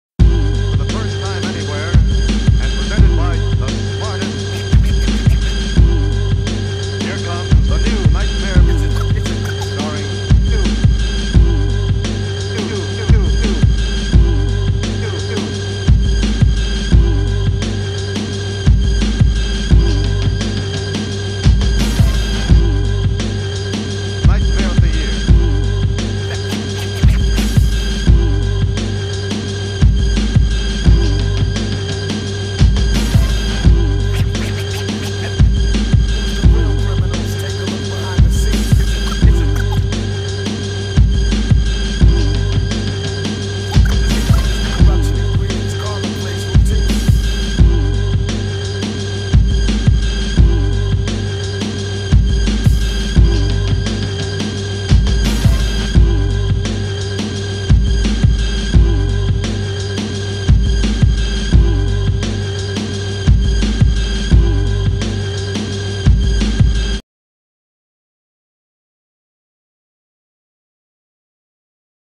Volcom Stone's Psychic Migrations, Carcavelos, Portugal Premiere
The Portuguese Psychic Migrations movie premiere went down in Carcavelos during the WSL surf contest last Tuesday, so a lot of the surfers starring in the movie were present for the delight of the fans. Full report here: